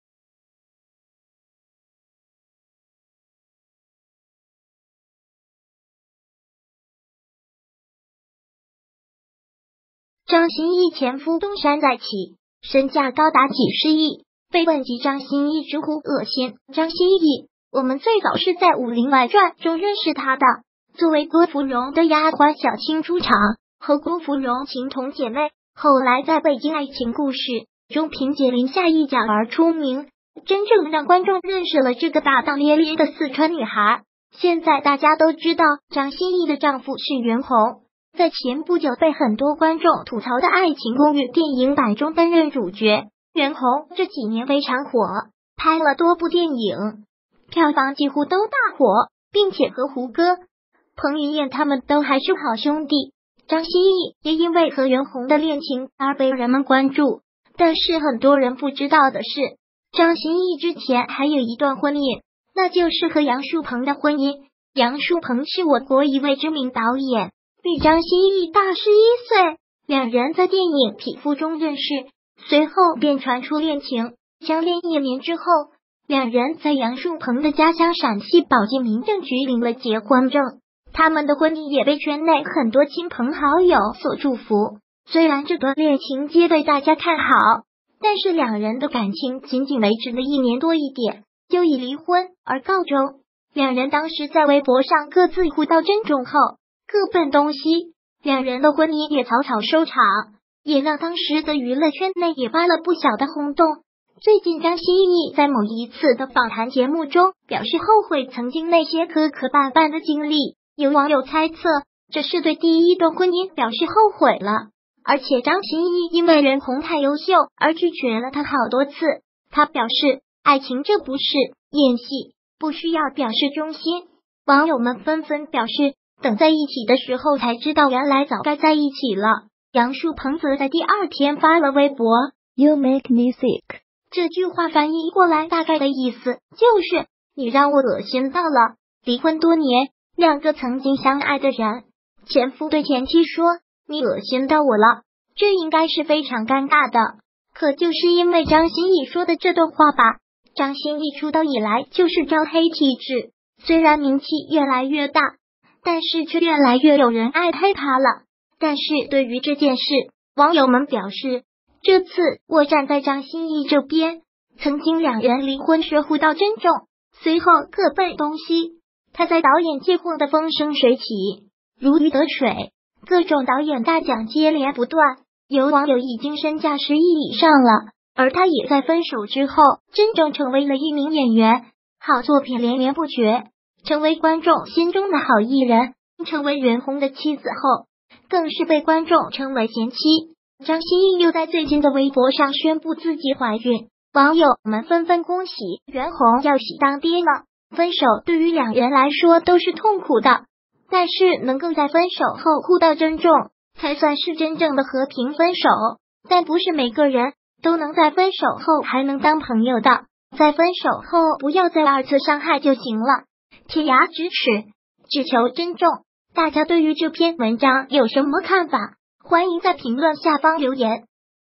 张歆艺前夫东山再起，身价高达几十亿，被问及张歆艺直呼恶心
张歆艺前夫东山再起，身价高达几十亿，被问及张歆艺直呼恶心张歆艺我们最早是在《武林外传》中认识她的，作为郭芙蓉的丫鬟小青出场，和郭芙蓉情同姐妹，后来在《北京爱情故事》中凭借林夏一角而出名，真正让观众认识了这个大大咧咧的四川女孩！现在大家都知道张歆艺的丈夫是袁弘，在前不久被很多观众吐槽的《爱情公寓》电影版中担任主角，袁弘这几年非常火，拍了多部电影，票房几乎都大火，并且和胡歌，彭于晏他们都还是好兄弟，张歆艺也因为和袁弘的恋情而被人们关注！但是很多人不知道的是，张歆艺之前还有一段婚姻，那就是和杨树鹏的婚姻！杨树鹏是我国一位知名导演，比张歆艺大11岁，两人在电影《匹夫》中认识，随后便传出恋情，相恋一年之后，两人在杨树鹏的家乡陕西宝鸡民政局领了结婚证，他们的婚姻也被圈内很多亲朋好友所祝福，虽然这段恋情皆被大家看好，但是两人的感情仅仅维持了一年多一点，就已离婚而告终，两人当时在微博上各自互道珍重后，各奔东西，两人的婚姻也草草收场，也让当时的娱乐圈内也发了不小的轰动!最近张歆艺在某一次的访谈节目中表示后悔曾经那些磕磕绊绊的经历，有网友猜测这是对第一段婚姻表示后悔了，而且张歆艺因为袁弘太优秀而拒绝了他好